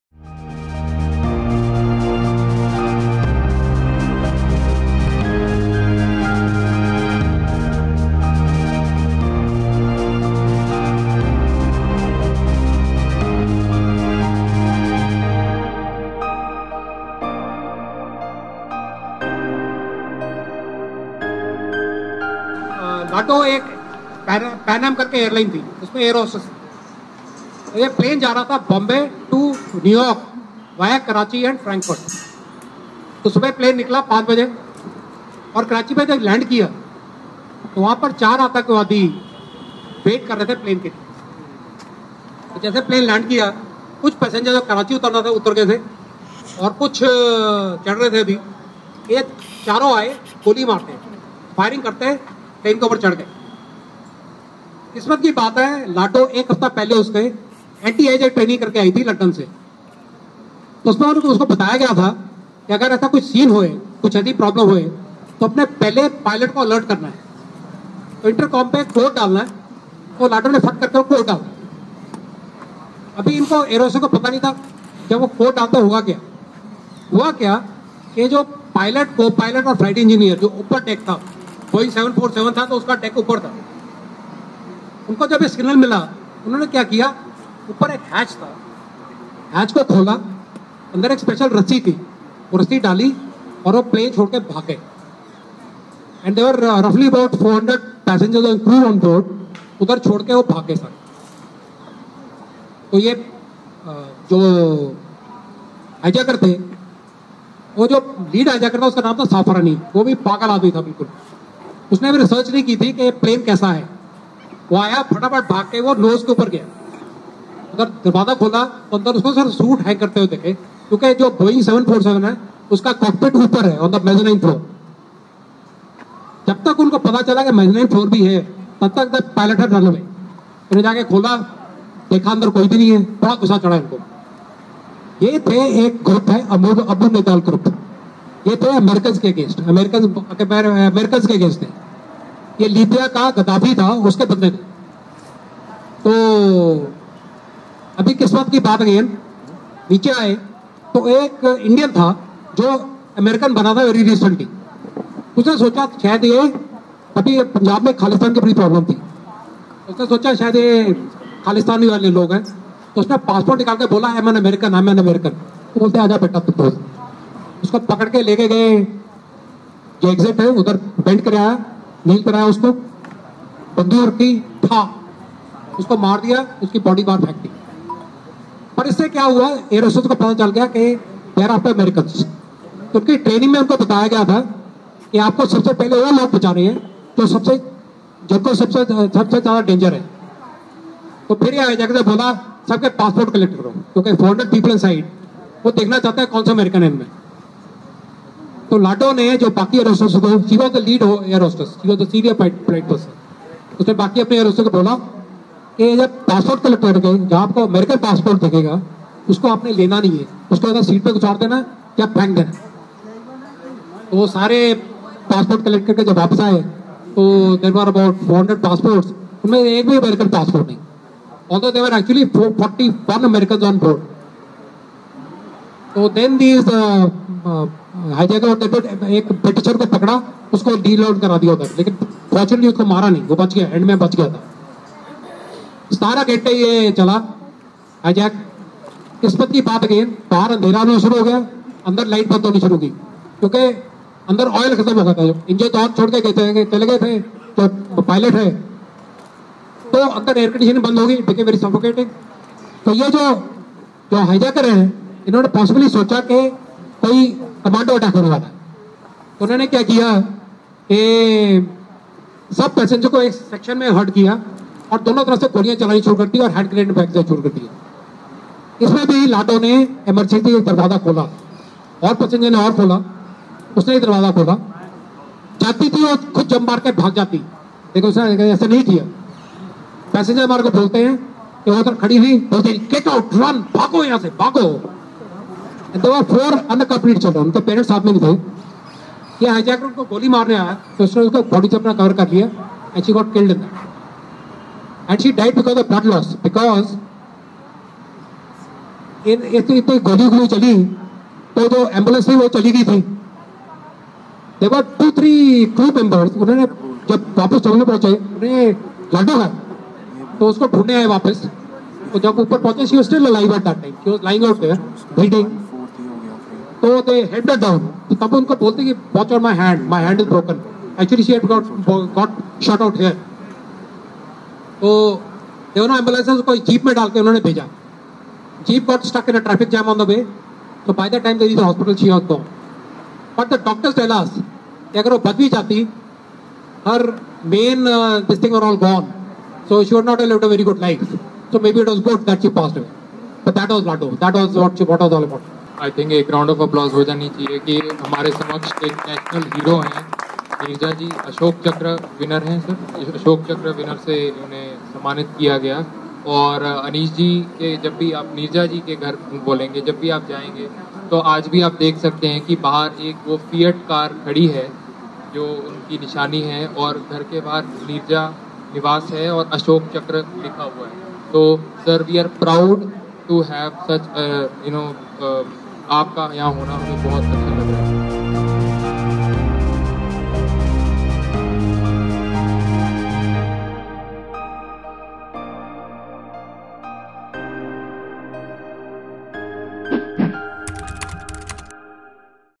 Rubaru Personality Talk ft. Aneesh Bhanot & Ojaswwee Sharma at TFT Chandigarh | RFE TV
Aneesh Bhanot talks about Shiv Kumar Sharma - an amazingly positive visually challenged person who says 'Nazar nahin, nazariye kee jaroorat hai'.

Watch Aneesh Bhanot in conversation with Ojaswwee Sharma on the third episode of InspiringIndians

Inspiring Indians by Aneesh Bhanot would see Aneesh Bhanot in conversation with Ojaswwee Sharma as they talk about about an Inspiring Indian Personality from the bestseller "Inspiring Indians Personalities Worldwide" published by United Newspapers (Chandigarh) authored by Aneesh Bhanot.

Genre - Talk Show/Lifestyle/Chat/Conversation

Producer: Ojaswwee Sharma